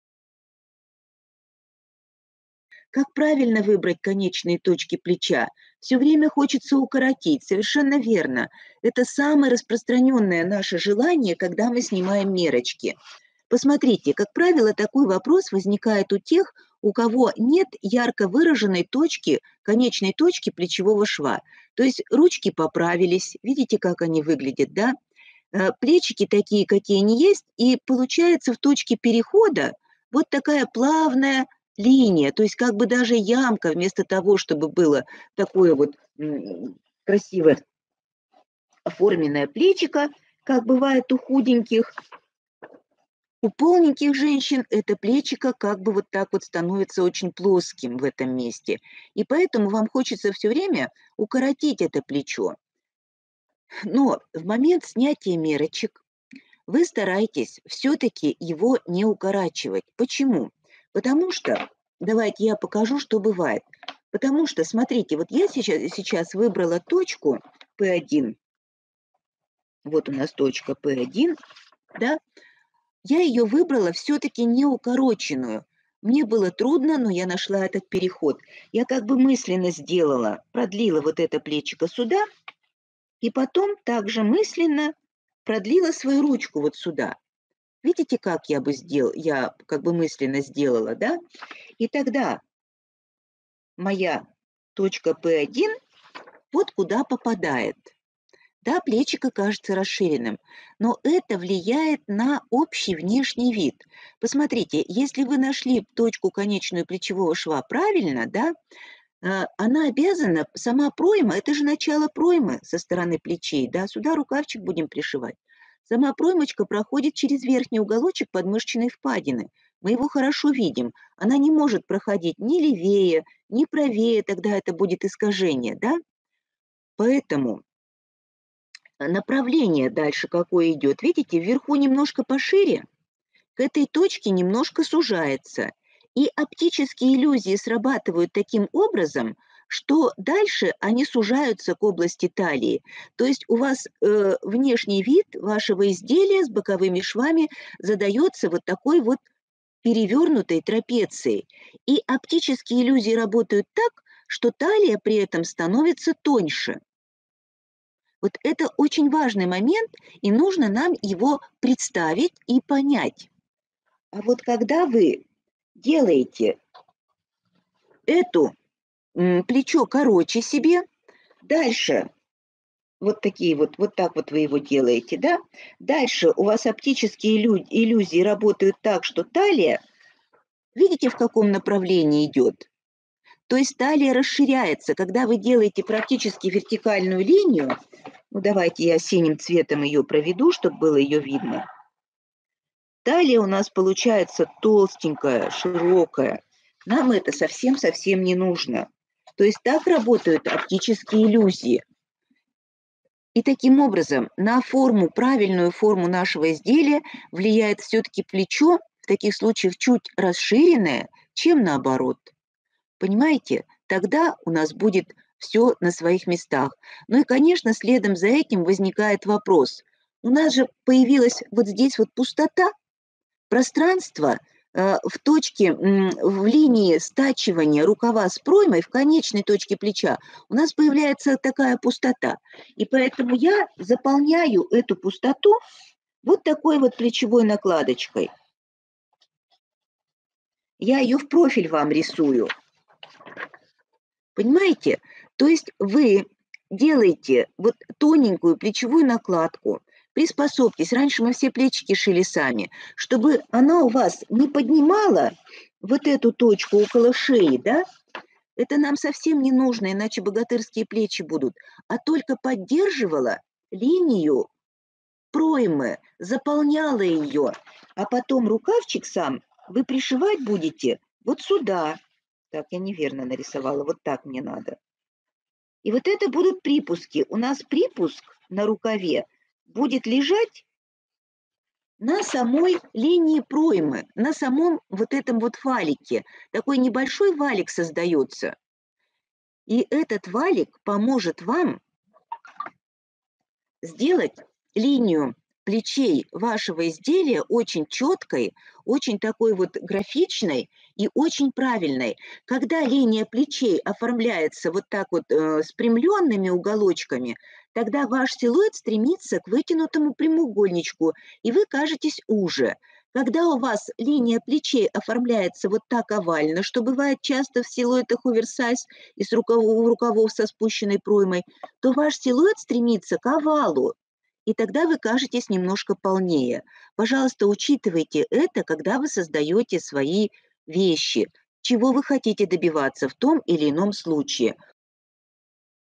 Очень частая ошибка. Линия плеча при полных руках. Шьем с Галиной Коломейко
Полные руки, линия плеча размыта. Хочется сделать плечо короче? Ни в коем случае не далайте этого. Положение конечной точки плеча может визуально расширить или сузить талию.
Смотрите видео до конца и не допускайте такой ошибки.
Дополнение: Не бойтесь подплечников! Времена громозких накладок давно прошли. Сейчас в продаже появились современные, тонкие и удобные.
А если в вашем городе таких не продают, то их не сложно сделать самим.